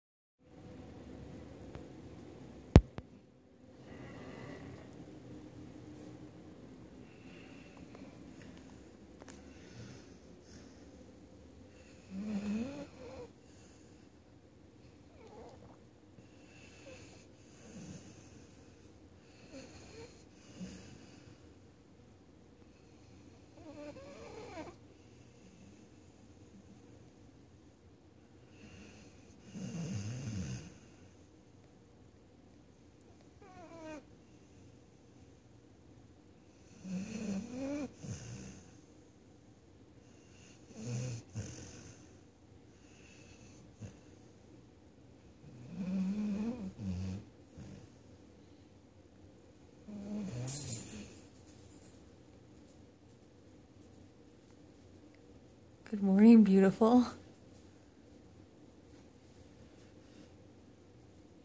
My Pit Bull Sleeps Weird...
Stella may not understand basic, everyday things (like pineapples or whistling). However, she has mastered the art of stealing my blanket and sleeping on the couch. And I suppose that's something. I am not sure if her mouth is too big or her tongue is too little, but she tends to do this a lot. As in, well, pretty much all the time. Skip ahead to 0:45 for the best part.

My dog is kind of internet famous: What We Can Learn From Stella, The Pit Bull Who Is Terrified Of Pineapples